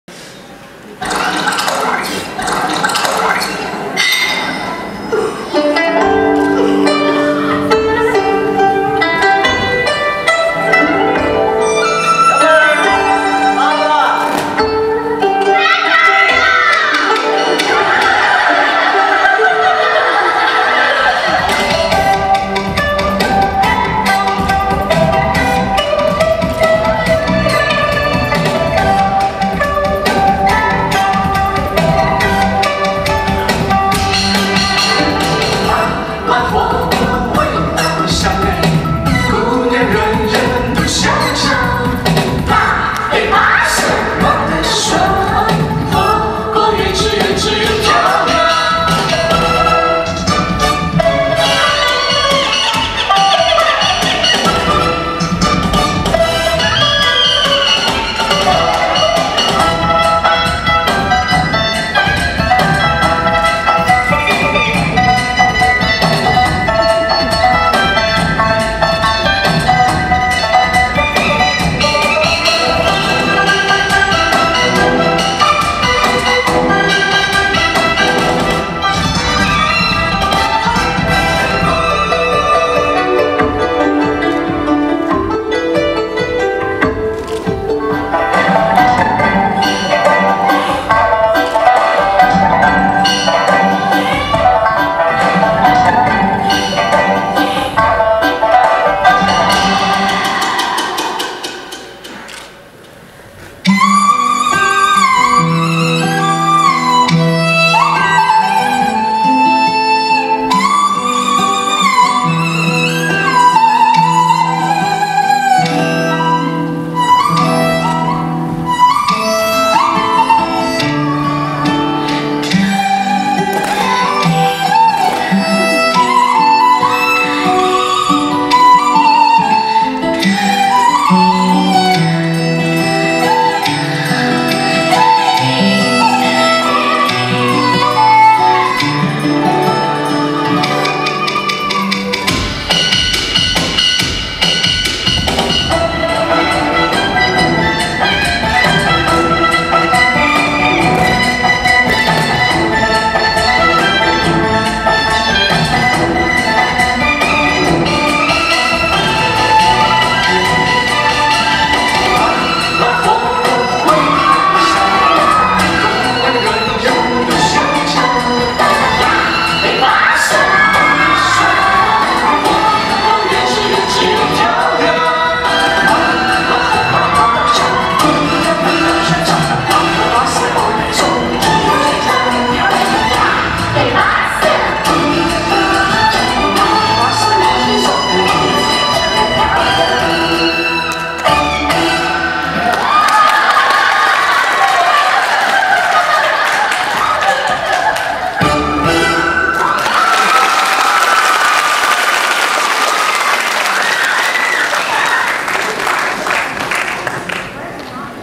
Spicy Hot Pot Girls CF22
From Best of ACPA performance at Ohlone College on June 11, 2017.